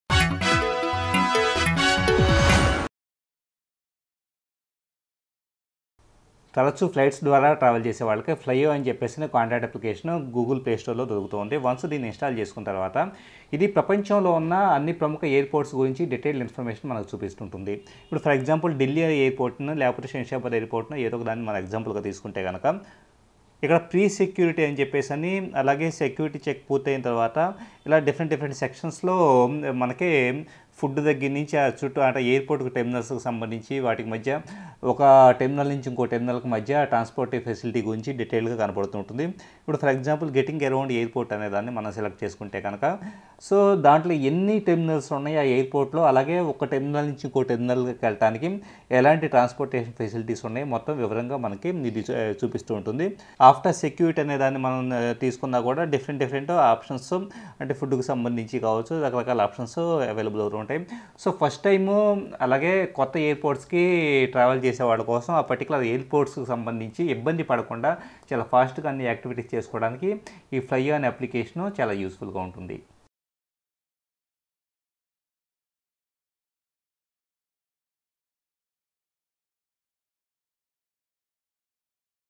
How to Know Complete Details of Any Airport with Android App?
People who travel to new places through flight comes across various new Airports. Having knowledge of every airport is essential for frequent traveler otherwise we have to face problems. In this context there is an Android app which provides detailed information about all the major international airports around the globe. Sridhar Nallamothu, Editor Computer Era magazine made a demo on this app.